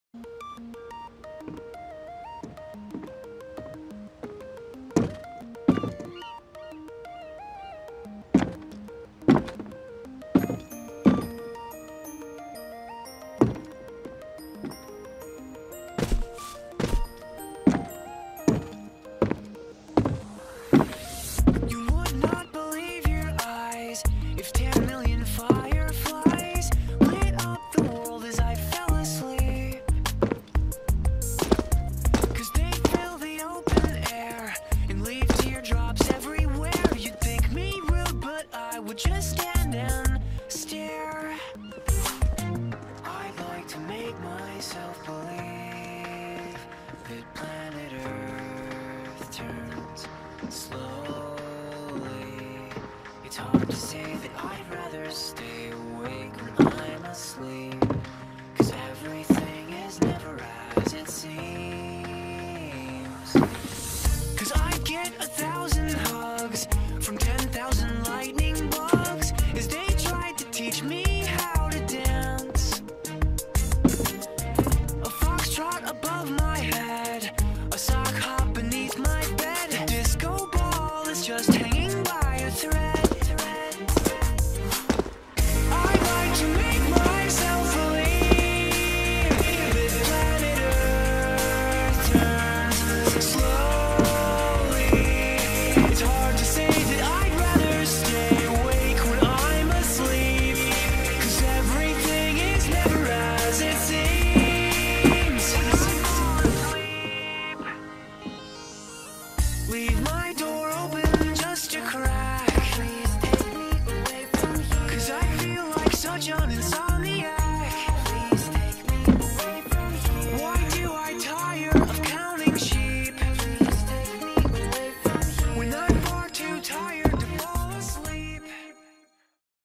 kz_pineforest_v2 in 2:06.58 SKZ
Done offline, so not on global database. Could be better but ive played this map for too long and im not running it anymore